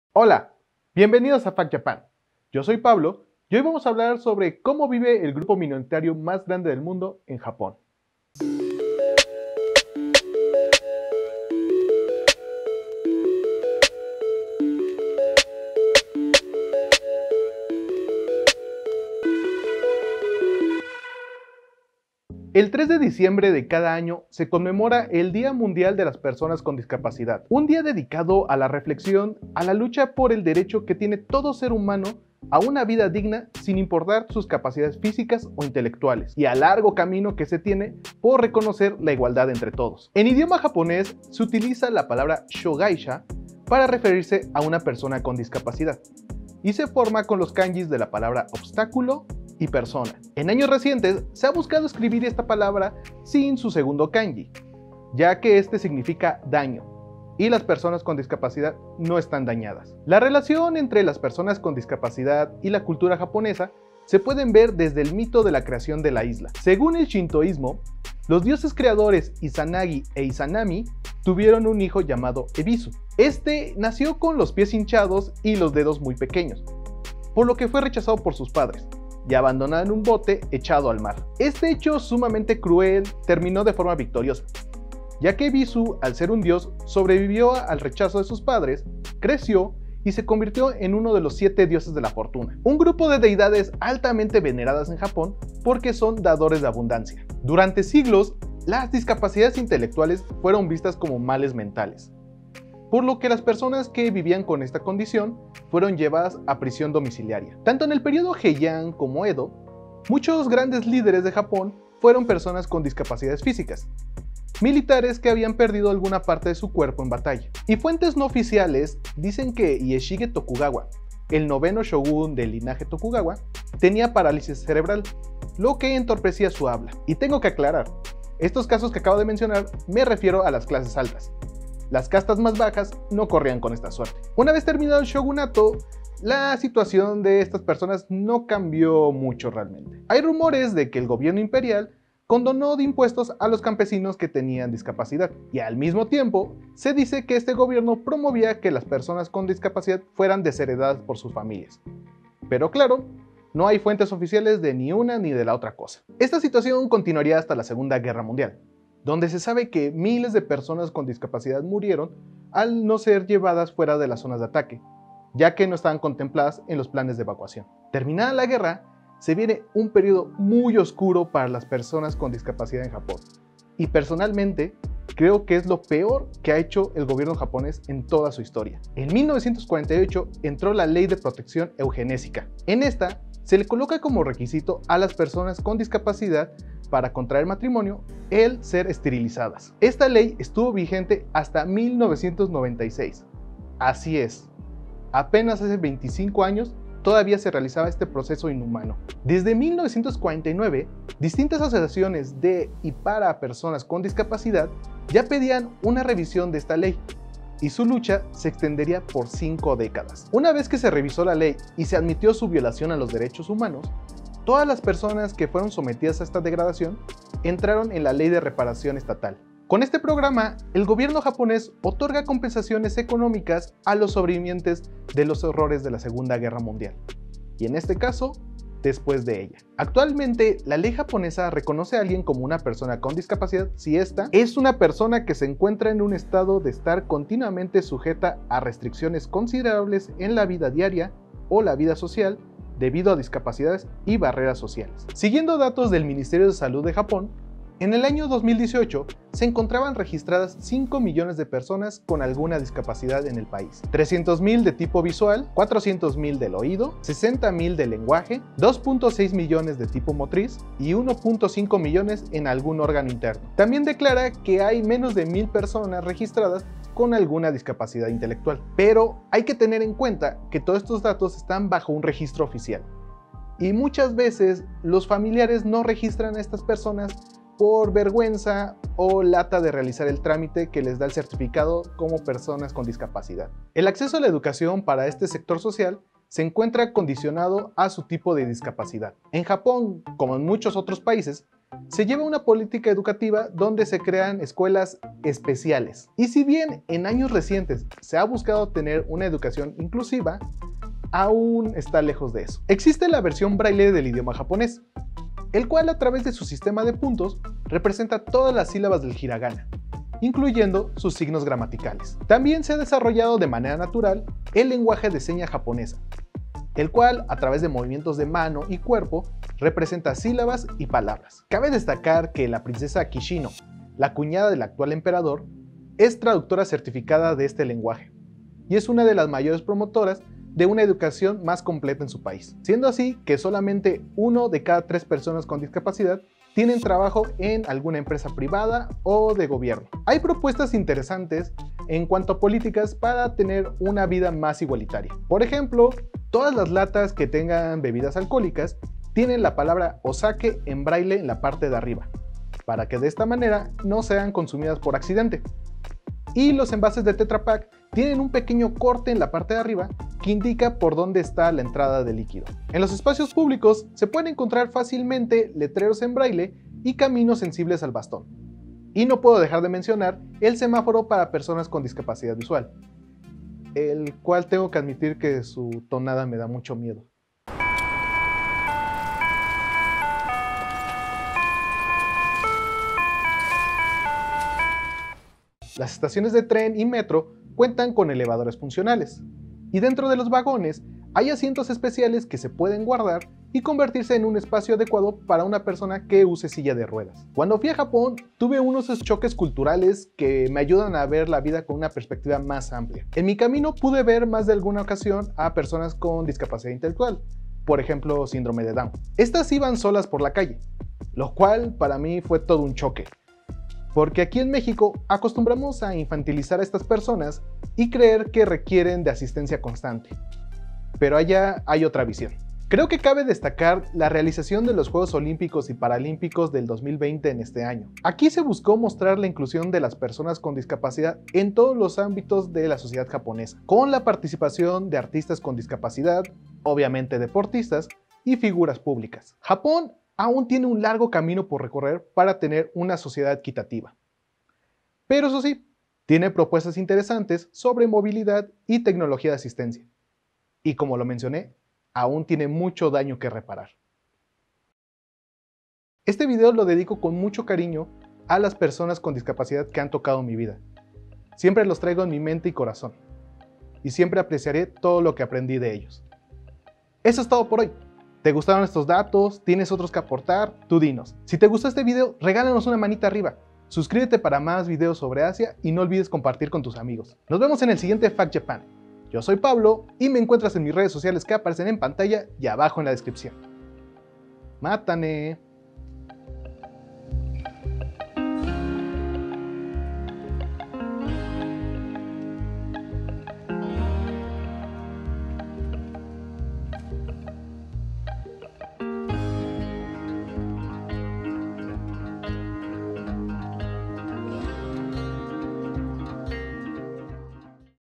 Historia y progreso de las personas con discapacidad en Japón ♿
"We have wings"

🎬 CRÉDITOS
Guión y locución: Pablo Millán
Producción y edición: Ángel Pardiñas
Ilustración y diseño: Jyuria Delgado
Corrección de estilo: Maya Asbeel